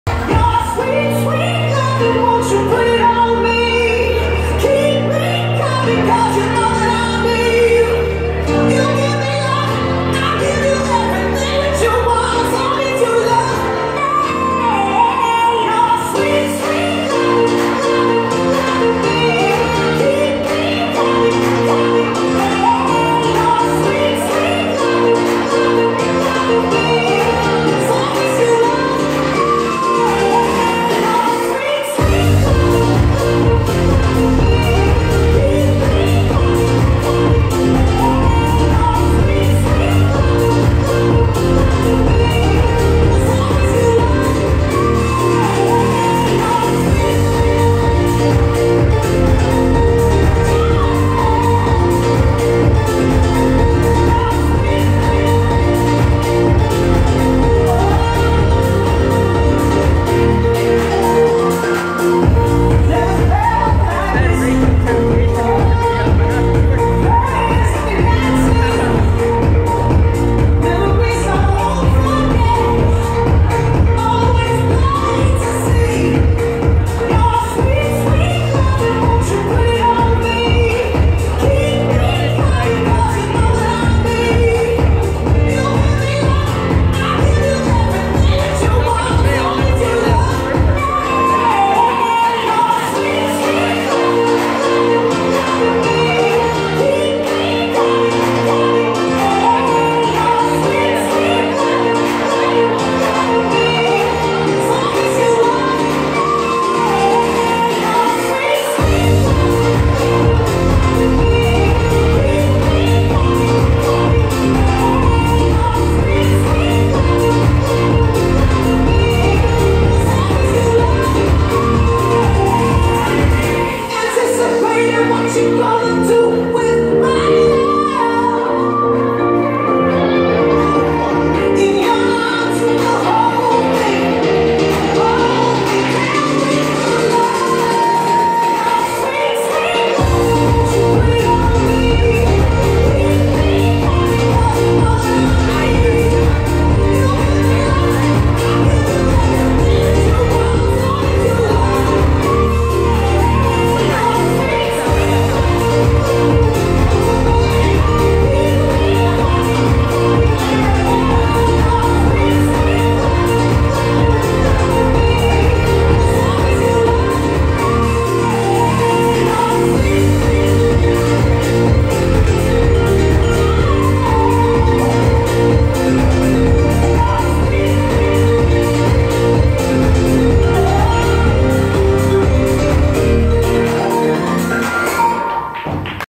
Sweet Loving with Rachael Temptation
On January 11 2019 Rachael Temptation performed in Oshawa at Club 717.